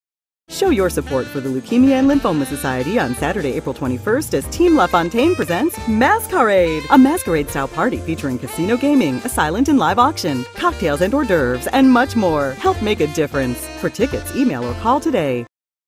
Team LaFontaine 2012 LLS Man & Woman of the Year Campaign Event | Mask-Car-Aid
Team LaFontaine Presents Mask-Car-Aid to benefit the 2012 Leukemia Lymphoma Society Man & Woman of the Year Campaign.

This Masquerade Style Party is our kick-off Fundraiser to support The Leukemia & Lymphoma Society. Join us for a Luxurious and Exciting Evening of Casino Gaming, Dueling Pianos, Silent & Live Auctions, Raffle Tickets for a Fantasy Vacation, Cocktails & Hors D'oeuvres, and so much more!!

Tickets are just $100 each which includes $200 in playing chips to use on the Casino Floor!

If you've never been to a LaFontaine FUN-Raiser, now's your chance! Masquerade style masks will be provided when you enter through Cadillac Showroom for complimentary valet parking.